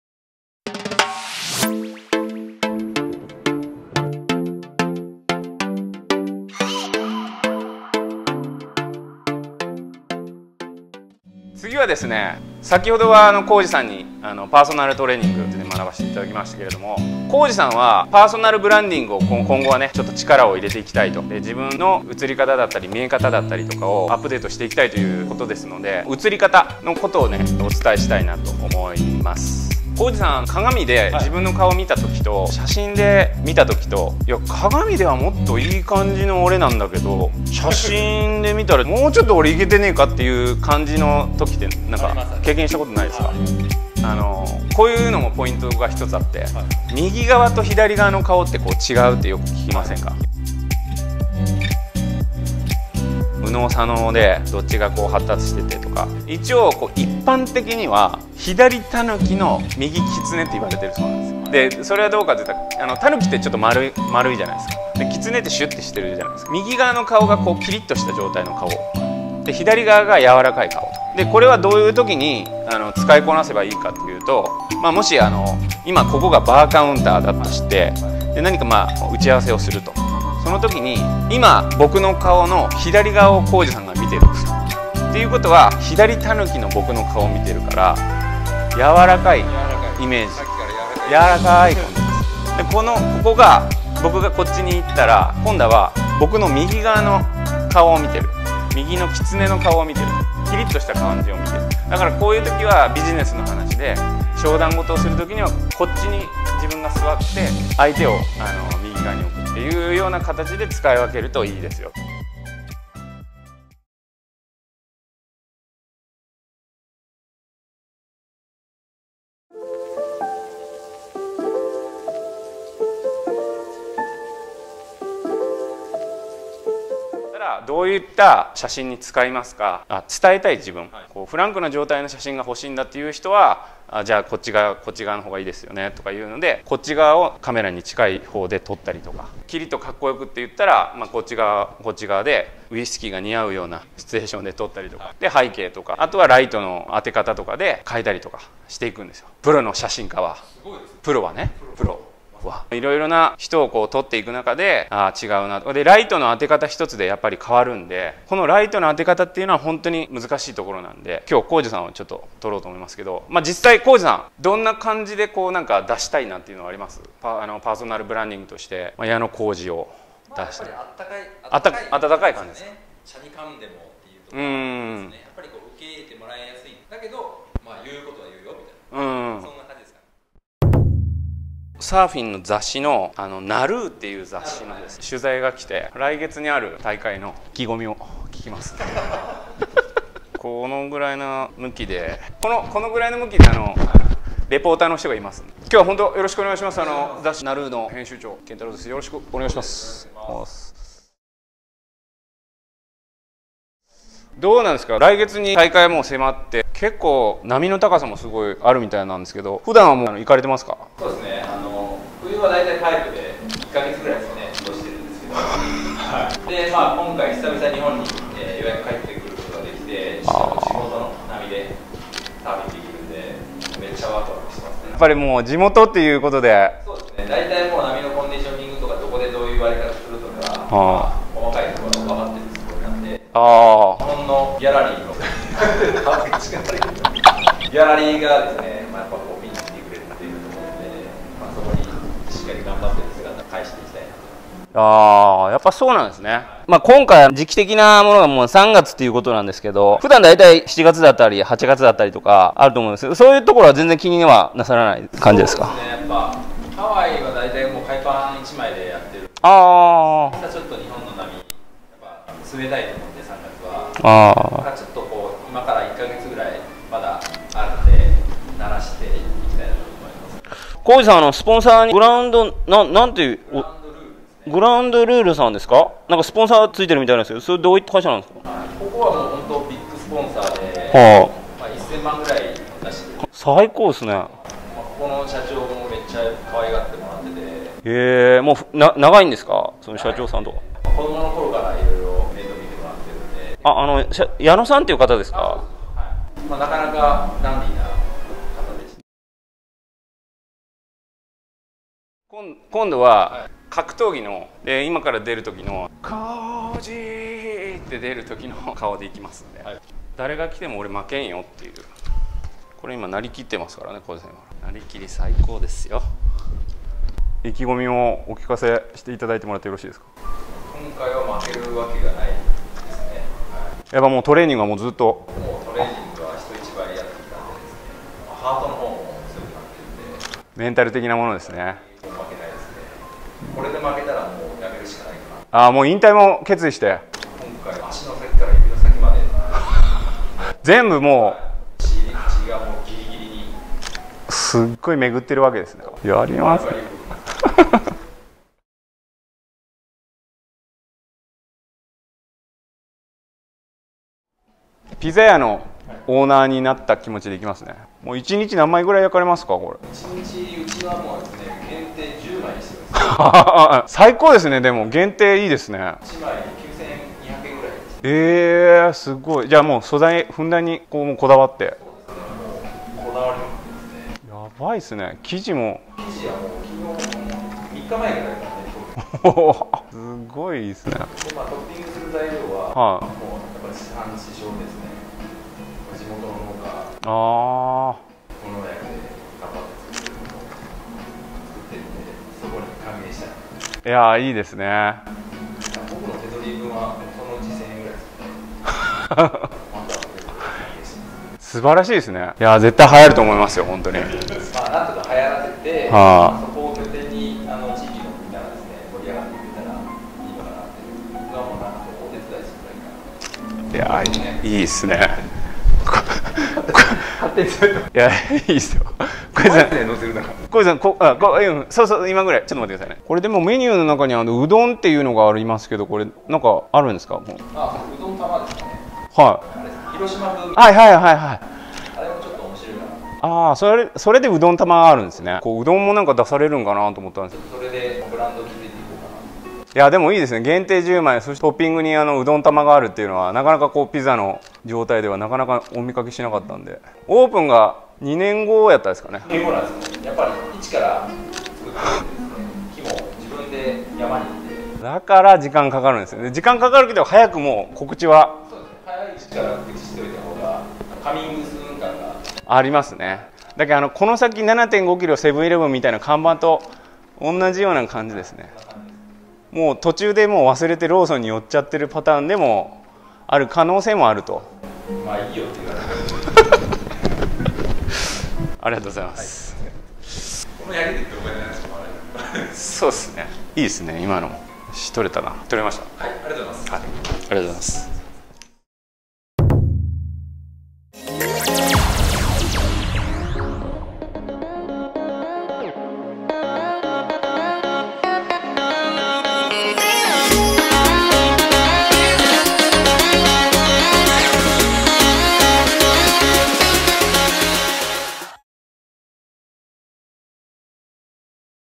プロフィール撮影の裏側！！あなたの利き顔は右それとも左？！！写真撮影
前回は、福岡県は北九州市八幡にある　ジムのオーナー　コージさんに来ていただいて自分の体の現在地を教えて頂きましたが

今回は、写真の写り方についてお話と実際に撮影をさせて頂きました。

宣材写真が必要な方、写真写りが良くなる方法、写真を撮られるのが苦手な人必見！！

こちらも良かったら観てみて下さい
【鏡に写った自分の方が写りが言い訳は？！】

前回の動画がこちら

健康な体作りに必要なことは、今の自分の状態を知ること！！

【コージさんのトレーニングジムのご案内】

 〒807-0831 福岡県北九州市八幡西区則松１丁目１０−３３